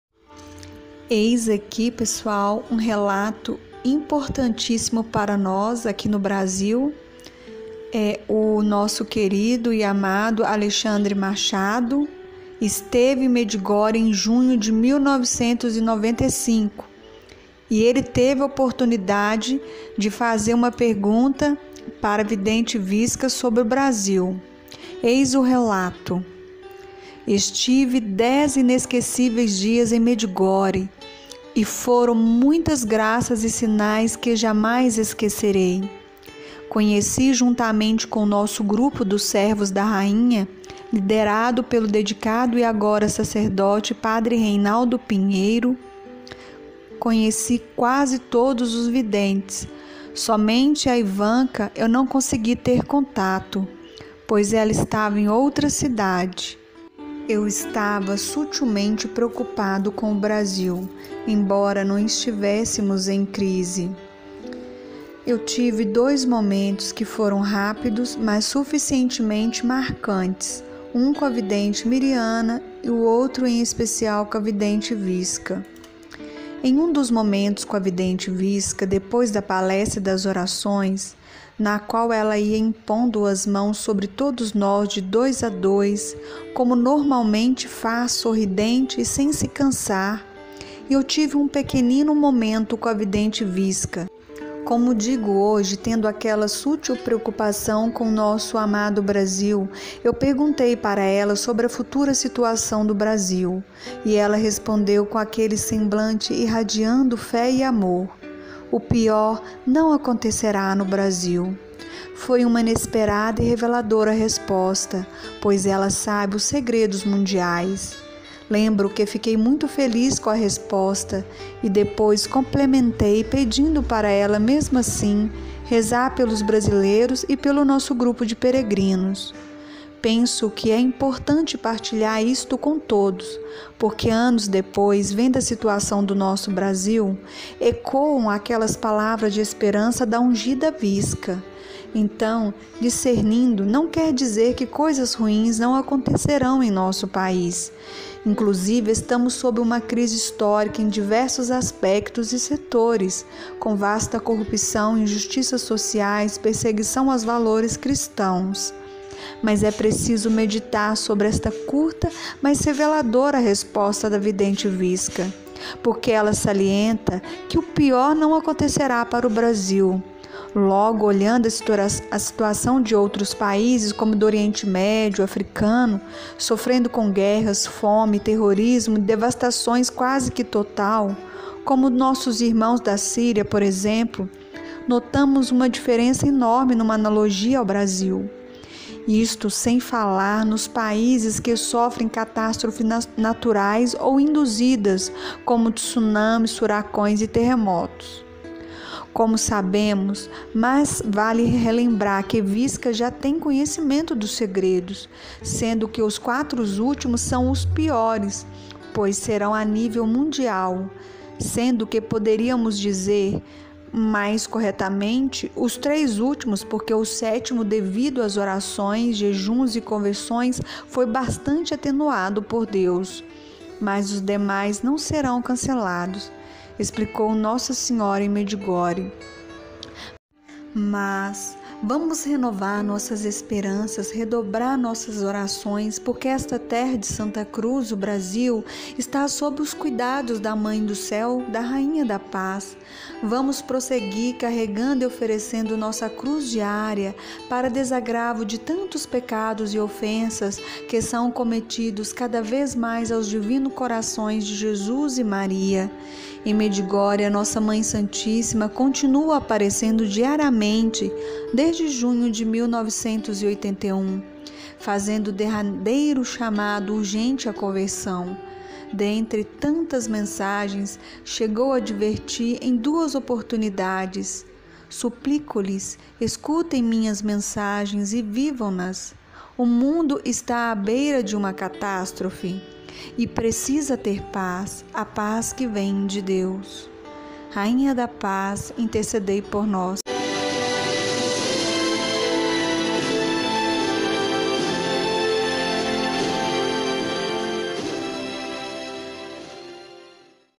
Medjugorje! O Pior Não Acontecerá No Brasil,Diz Vidente Vicka.
VEJAM O VÍDEO! Medjugorje! O Pior Não Acontecerá No Brasil, diz Vidente Vicka.
Relato do Nosso Querido e Amado Alexandre Machado.
Retornando ao ponto, depois da palestra e das orações impondo as mãos sobre todos nós, de dois a dois, como normalmente faz sorridente e sem se cansar, eu tive um pequenino momento com a Vicka, como digo hoje, tendo aquela “sutil preocupação com nosso amado país”, e perguntei para ela sobre “a futura situação do Brasil” e ela respondeu com aquele semblante irradiando fé e amor: “O pior não acontecerá no Brasil…”  Foi uma inesperada e reveladora resposta, pois ela sabe os segredos mundiais!
Lembro que fiquei muito feliz com a resposta e depois complementei pedindo para ela, mesmo assim, “rezar pelos brasileiros e nosso grupo de peregrinos.
Compartilhem! Aperte o Sino de Notificações! Deixe o seu comentário e o seu Like!